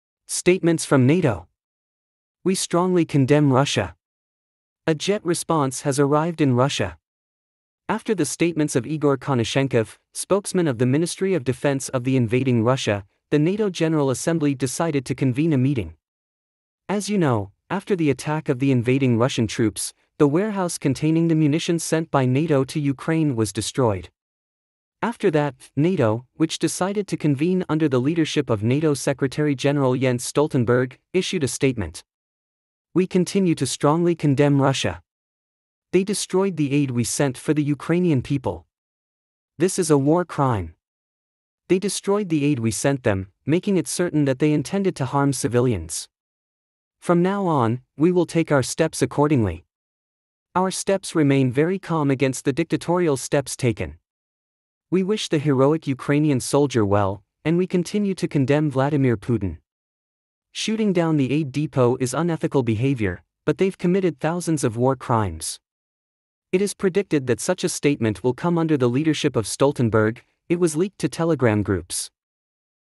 5 Minutes Ago! Nato is on the Move! The Invading Russians Will Pay For It
Statements from NATO! We strongly condemn Russia! A jet response has arrived in Russia! After the statements of Igor Konashenkov, spokesman of the Ministry of Defense of the invading Russia, the NATO general assembly decided to convene a meeting! As you know, after the attack of the invading Russian troops, the warehouse containing the munitions sent by NATO to Ukraine was destroyed. After that, NATO, which decided to convene under the leadership of NATO Secretary General Jens Stoltenberg, issued a statement. “We continue to strongly condemn Russia. They destroyed the aid we sent for the Ukrainian people. This is a war crime. They destroyed the aid we sent them, making it certain that they intended to harm civilians. From now on, we will take our steps accordingly. Our steps remain very calm against the dictatorial steps taken. We wish the heroic Ukrainian soldier well, and we continue to condemn Vladimir Putin. Shooting down the aid depot is unethical behavior, but they've committed thousands of war crimes.” It is predicted that such a statement will come under the leadership of Stoltenberg, it was leaked to telegram groups.